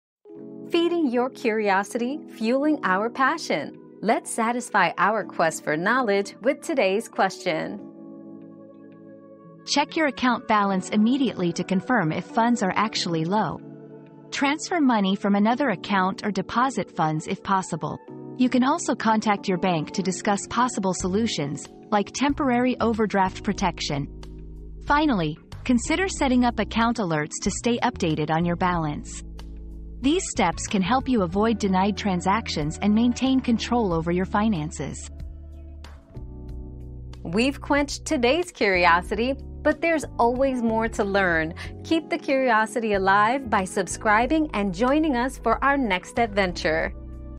What to do when your debit card says insufficient funds?
Got Insufficient Funds on Your Debit Card? Here's What to Do 👉 Debit Card Insufficient Funds 👉 Learn quick and effective steps to handle insufficient funds on your debit card, including checking your balance, transferring money, contacting your bank, and setting up account alerts.

Laura S. Harris (2024, June 23.) What to do when your debit card says insufficient funds?
    🌐 AskAbout.video